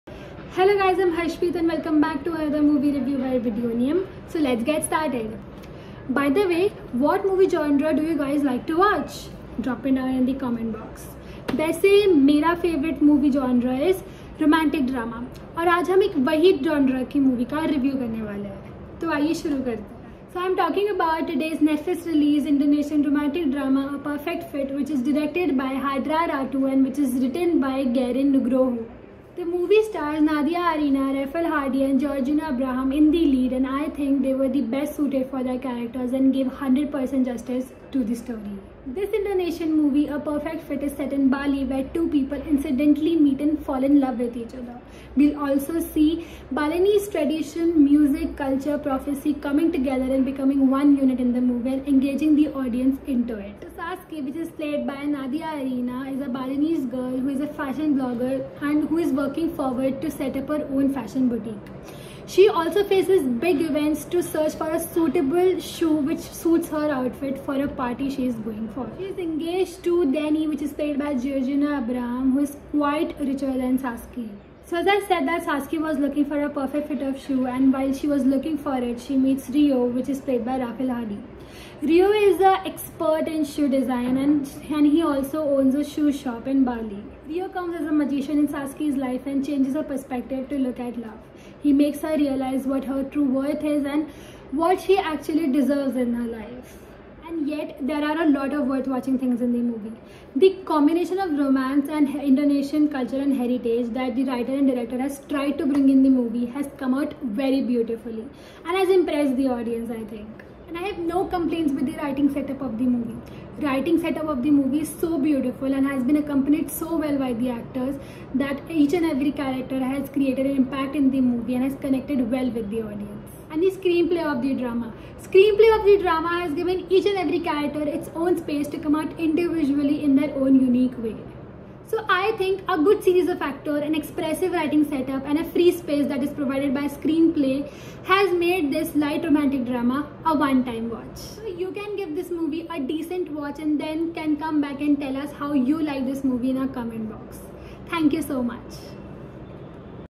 A Perfect Fit Movie Review | Indonesian Movie on Netflix | July 15, 2021 | Videonium
Today, Videonium Anchor Harshpreet Kaur reviews the Indonesian romantic movie "A Perfect Fit."

Director: Hadrah Daeng Ratu
Writer: Garin Nugroho (screenplay)
Starring: Nadya Arina, Refal Hady, Giorgino Abraham, and others

Please visit the "About" section of our YouTube channel to email your business-related queries to us.